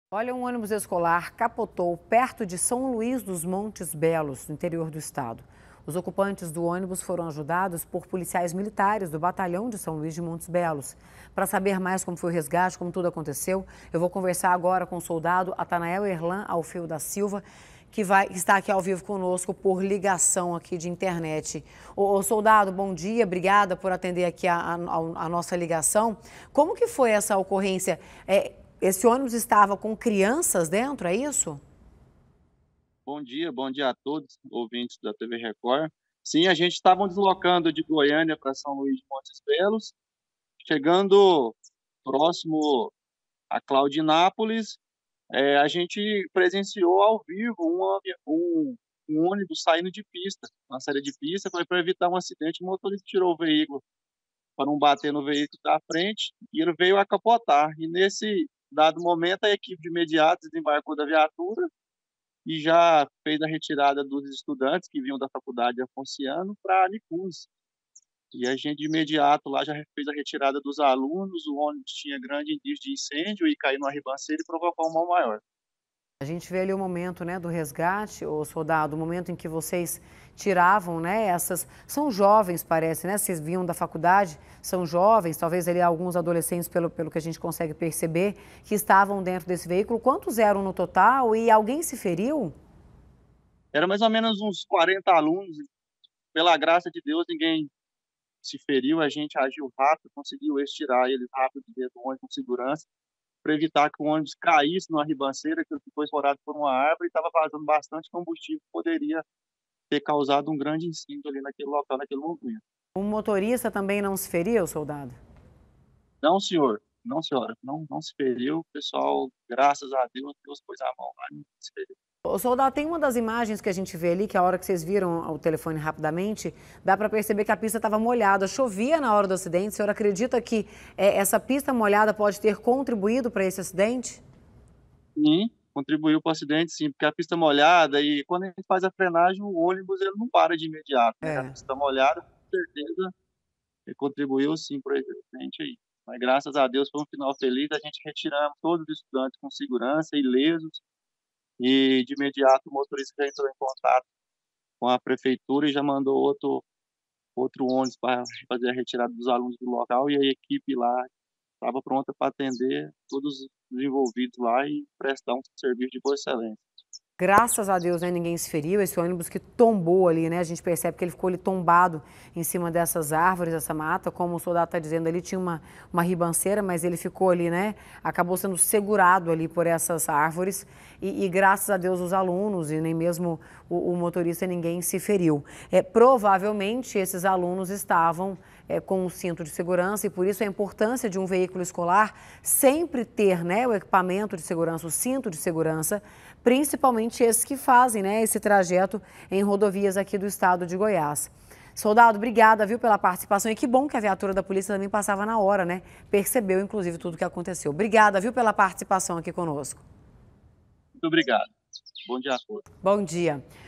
ACIDENTE COM ÔNIBUS ESCOLAR: VEÍCULO CAPOTOU EM RODOVIA
UM ÔNIBUS ESCOLAR CAPOTOU PERTO DE SÃO LUIS DOS MONTES BELOS. OS OCUPANTES DO ÔNIBUS FORAM AJUDADOS POR POLICIAIS MILITARES DO BATALHÃO DE SÃO LUIS DE MONTES BELOS. PARA SABER COMO FOI O RESGATE, EU CONVERSO AGORA COM O SOLDADO ATANAEL ERLAN ALFEU DA SILVA